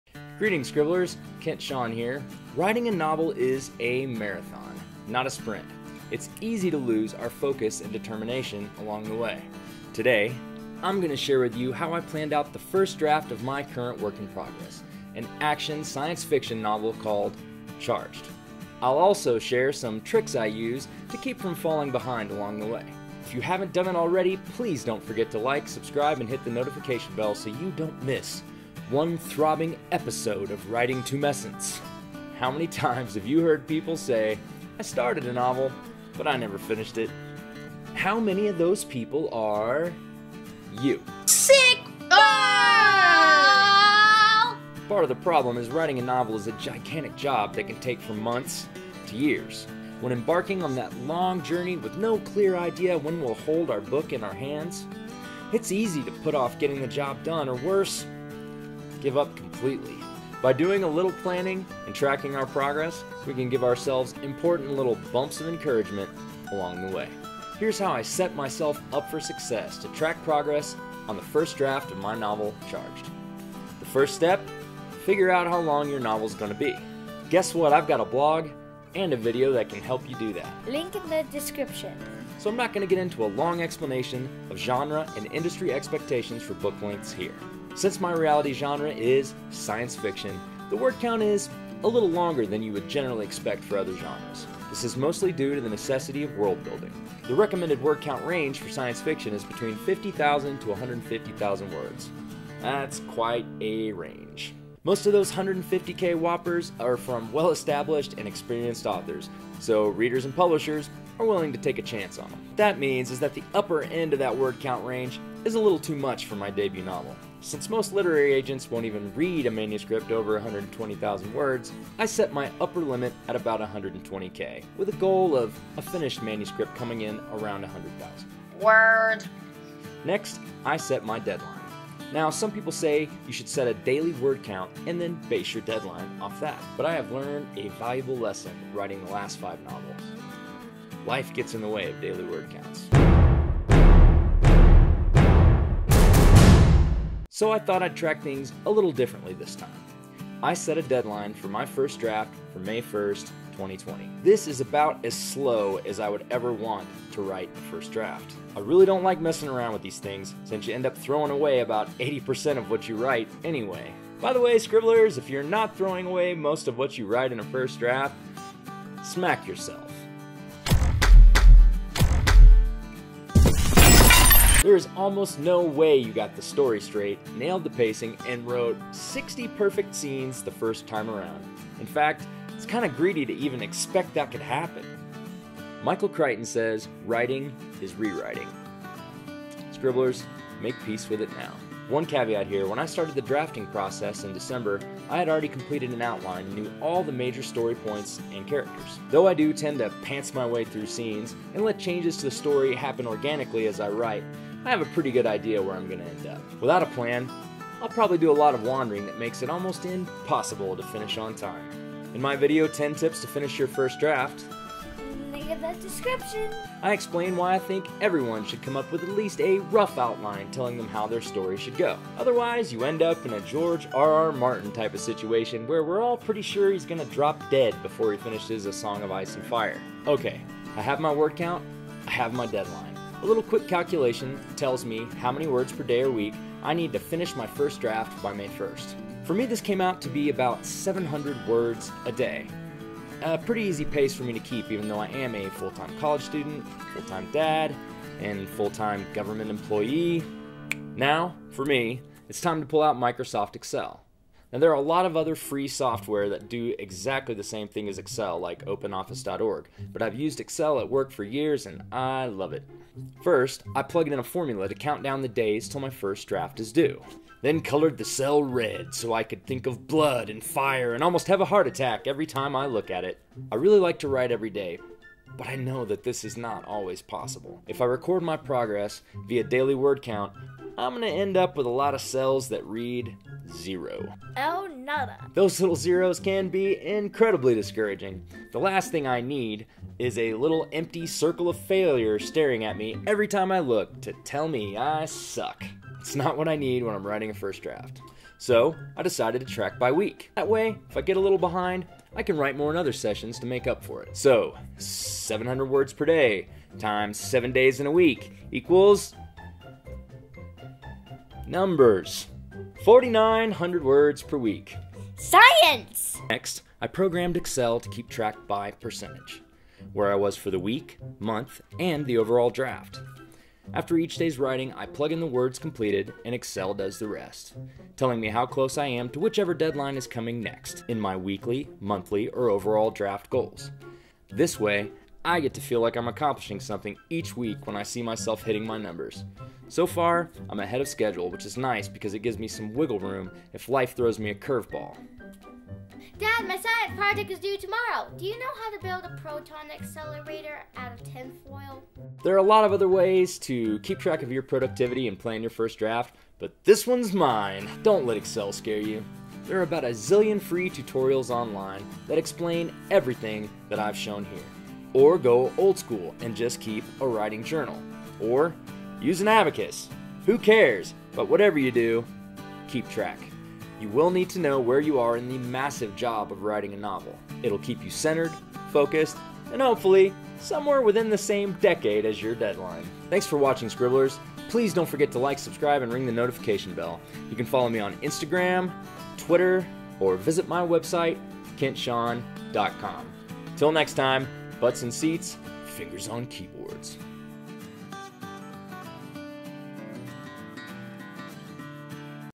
Track Your Progress Like a Boss, Kent Shawn Style! (AUTHORTUBE)
Self bludgeoning, abacus use, and tin foil proton accelerators await in this week's exciting video! Learn how I track progress on the first draft of my debut self-published novel, CHARGED.

Have a suggestion for a new video? Leave it in the comments below.